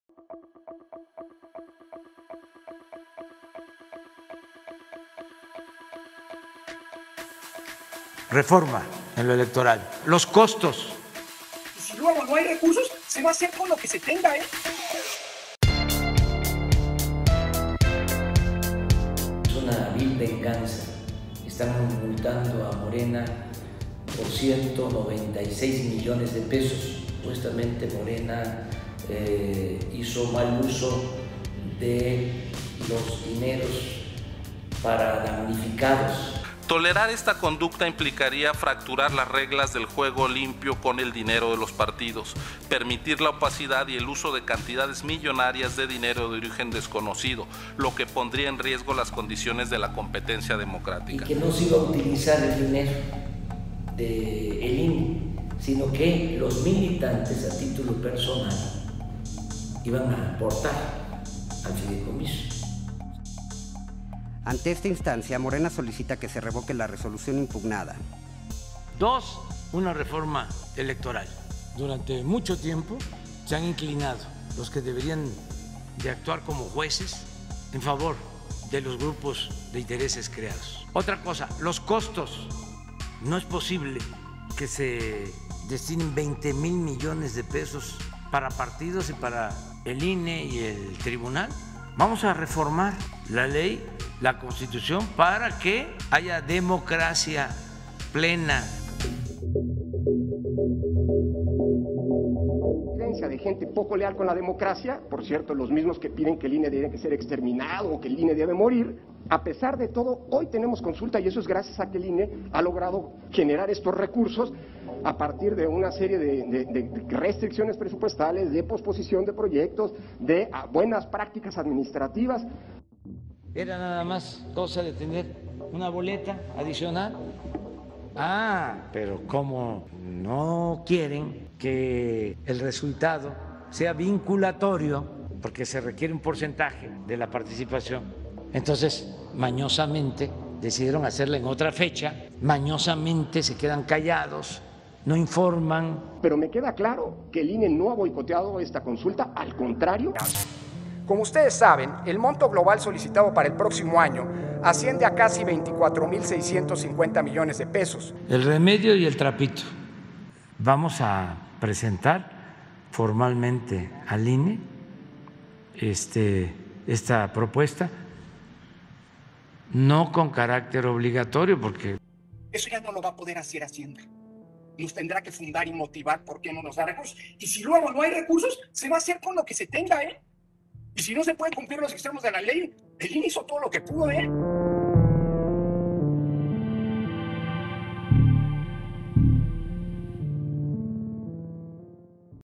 AMLO vs INE
La confrontación entre el presidente Andrés Manuel López Obrador, Morena y diversos personajes de la “4T” en contra el Instituto Nacional Electoral (INE) ha sido una constante por diversos temas.